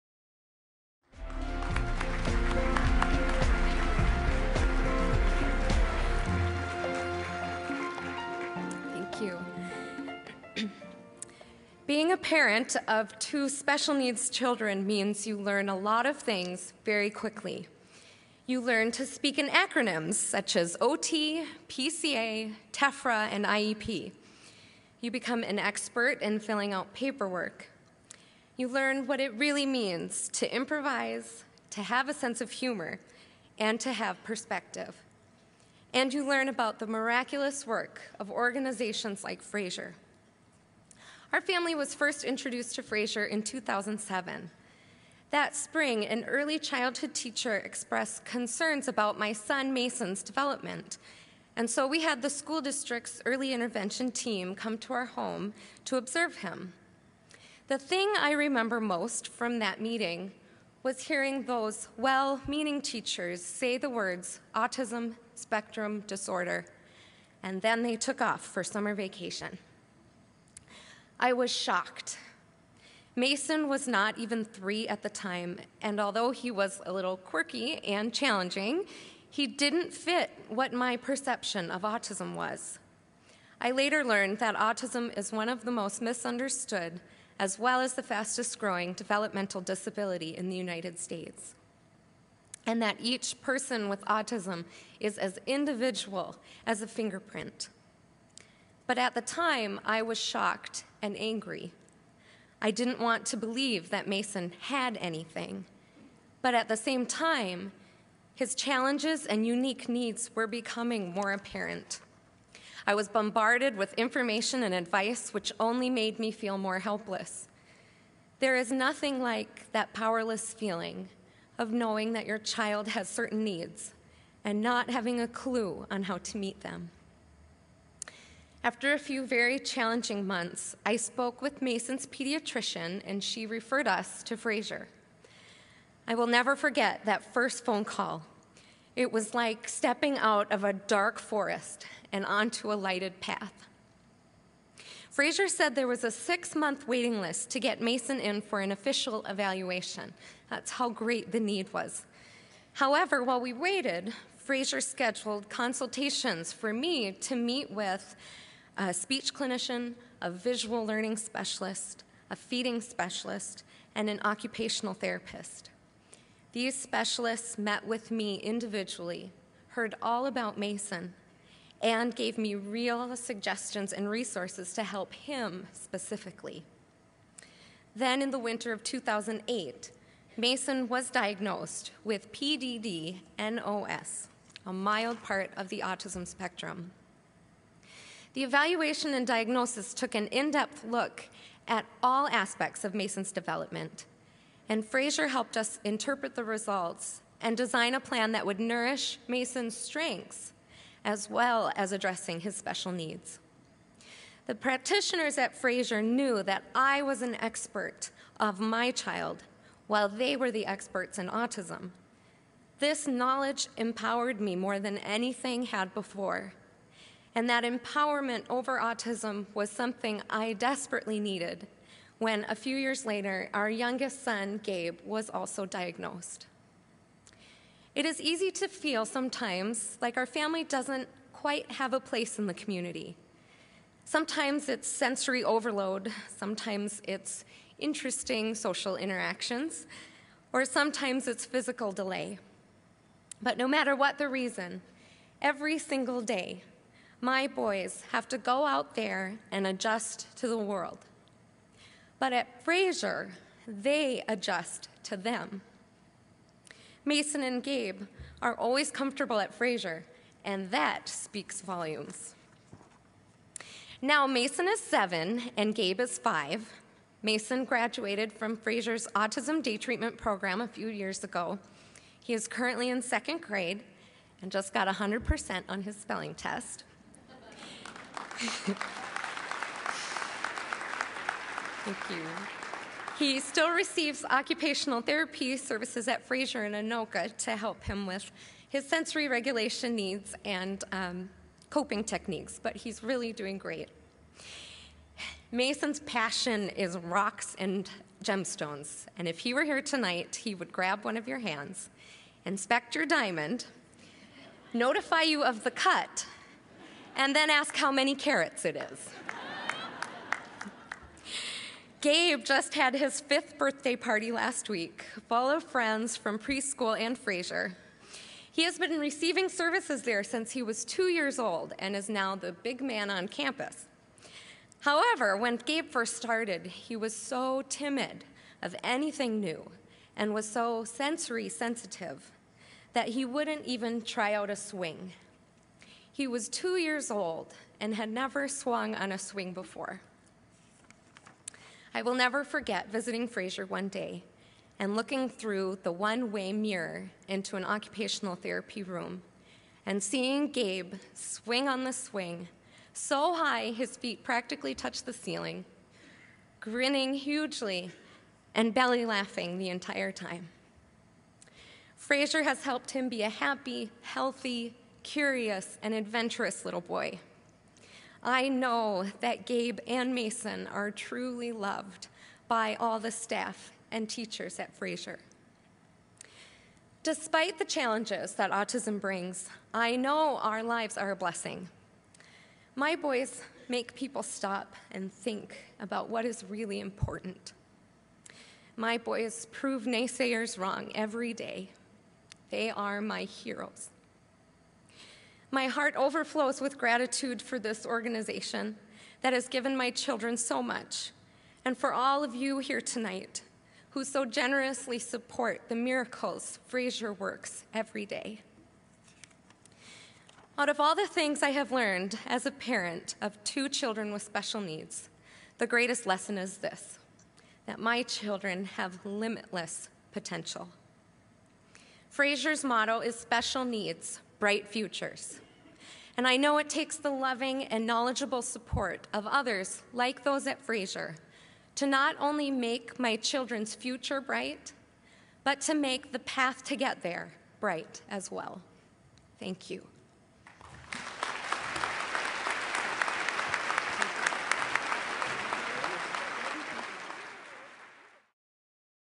Amy Oelkers speaks at the Fraser Annual Benefit 2011
Fraser parent Amy Oelkers give the keynote speech at the Fraser Annual Benefit in Minneapolis on November 19, 2011. She shares the story of discovering that her two boys have autism, and the difference that Fraser made in her family's life.